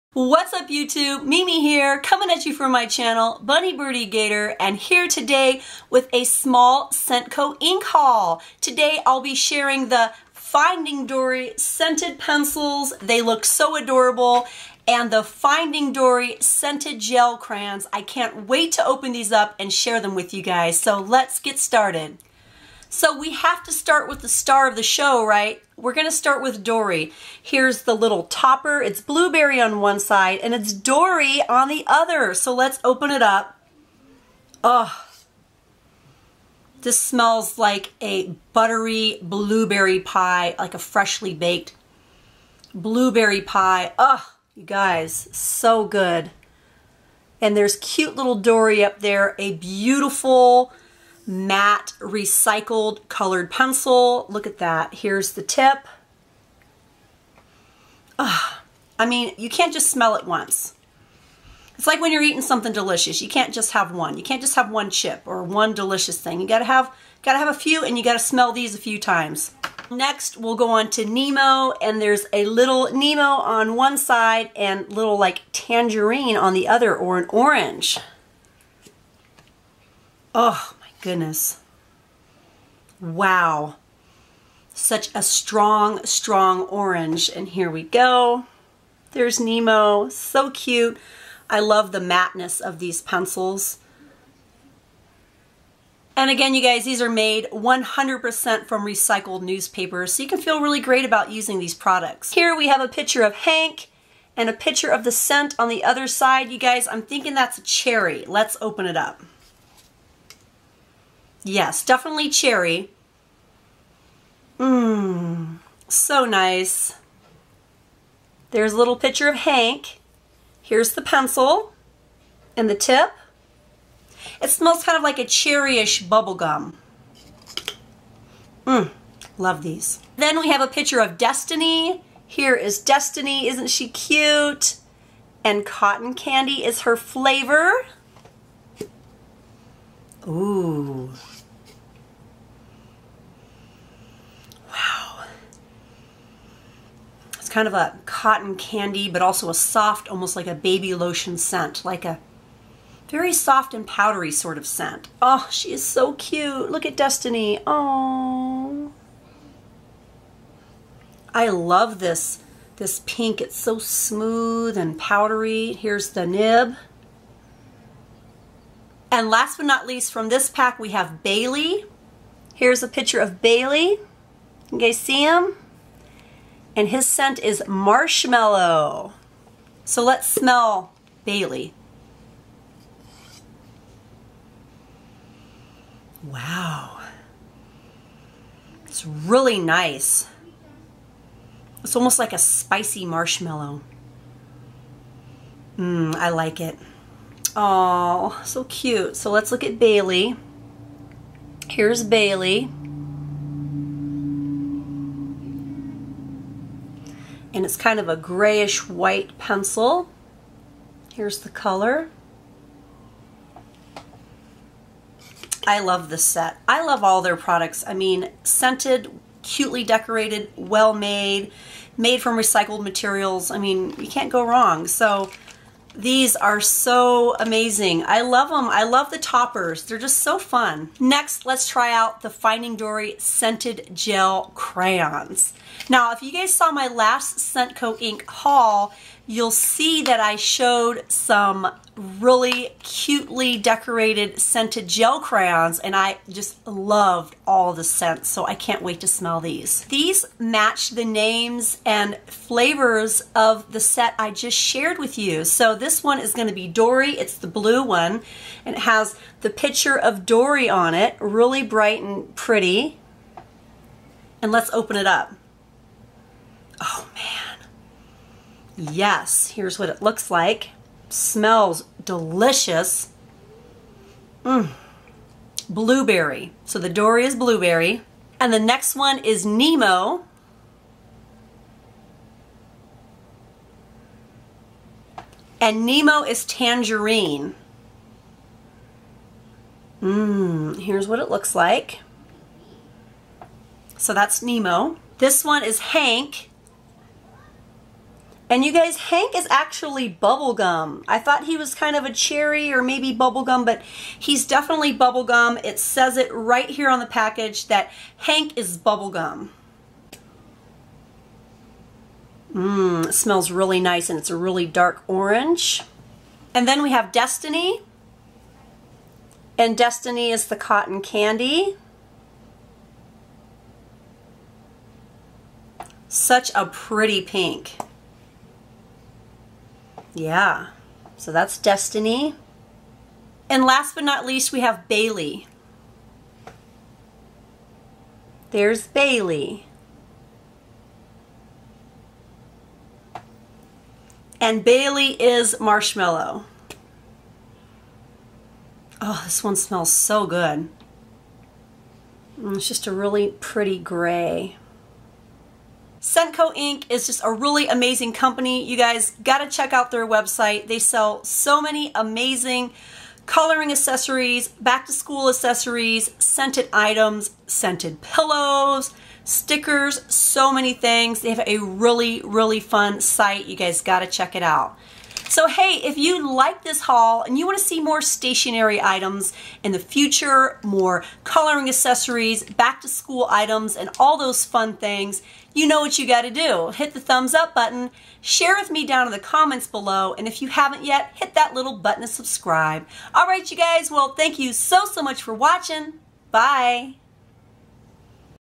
FINDING DORY | Scented Colored Smencils & Scented Gel Crayons | SCENTCO INC.
Check out these super scented FINDING DORY Scented Colored Smencils & Scented Gel Crayons from SCENTCO INC.!!

Products showed:
Disney Pixar Finding Dory: Stencils 5 Pack
Disney Pixar Finding Dory: Gel Crayons 5 Pack

I am not being paid by the mentioned companies to make this video.